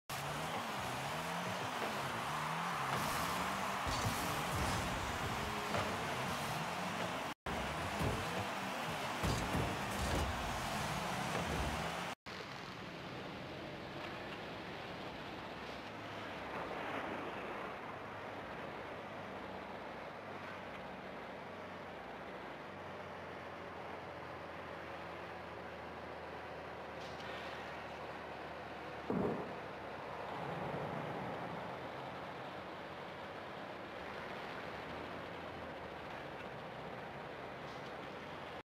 Save
Not the most difficult save, but it was clutch (this was one of my 6 saves in this game).  This is game 2 of the finals of a champion tournament.  We had lost the first game 1-0 (on an own goal by a teammate).  We went on to win in OT and then we easily won game 3 to win the tournament.